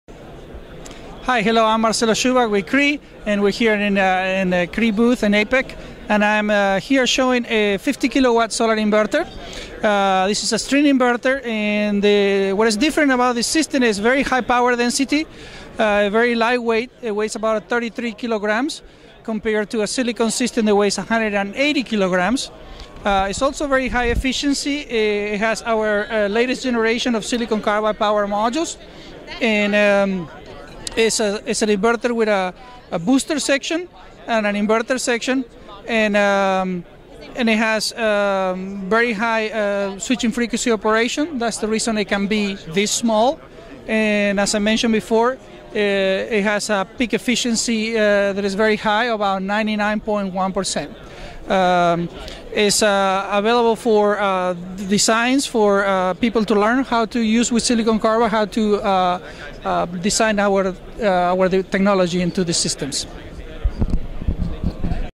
Cree shows a solar inverter using SiC technology at APEC 2015
In this video Cree shows an advanced solar inverter design using Cree Silicon Carbide (SiC) power semiconductors for Power Systems Design at APEC 2015. SiC MOSFET technology enables reduced size, weight and cost for PV inverters, from 20 to 50% when compared with silicon, while at the same time maintaining or increasing efficiency.